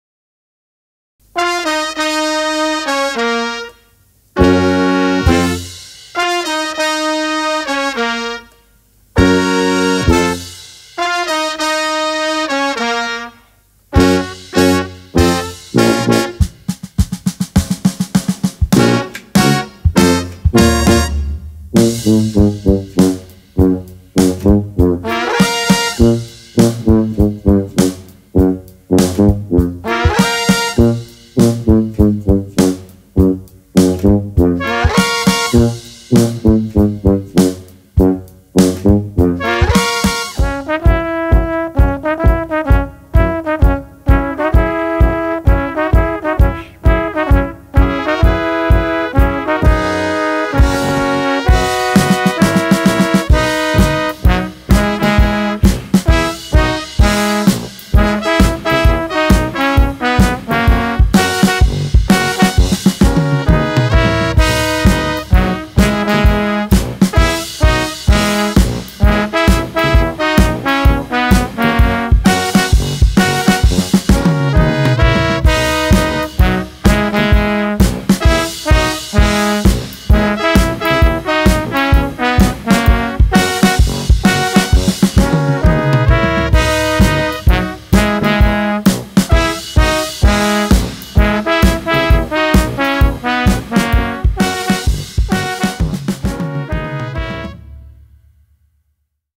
Belles of Brass - Oktoberfest - ELM Agency
Belles of Brass  offer a very entertaining and highly popular Oktoberfest Set, in which they wear traditional costumes, perform Bavarian tunes and rock anthems, and host interactive and immersive games with the audience.